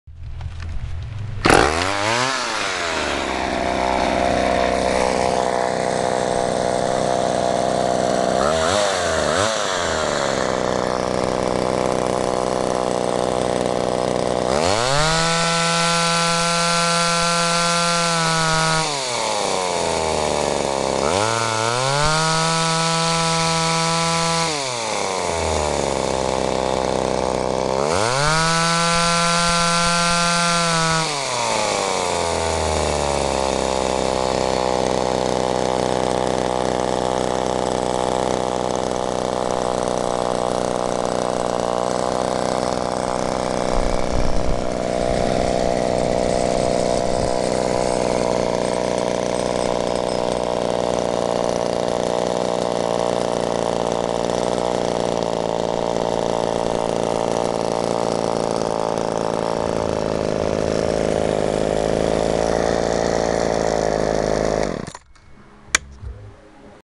Sears Craftsman Limited Edition XXV 2.0/14 Chainsaw Test Run
Rare Craftsman Limited Edition XXV 2.0/14 Chainsaw , start up and runs very good. Chain oiler works well , missing carb cover.